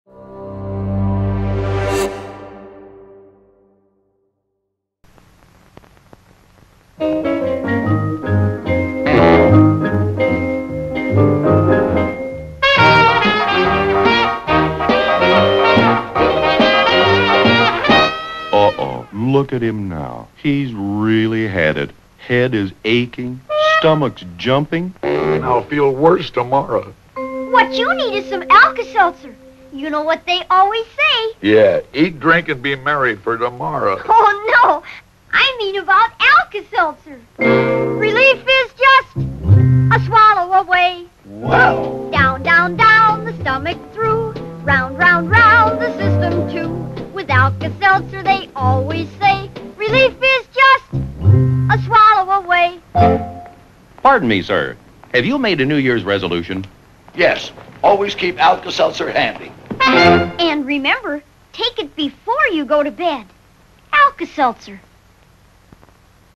Buster Keaton - Living It Up - Alka-Seltzer Commercial - Best Quality - Ye Old Movie Vault
Here's another classic Buster Keaton commercial for Alka-Seltzer in which he plays the part of a man living it up.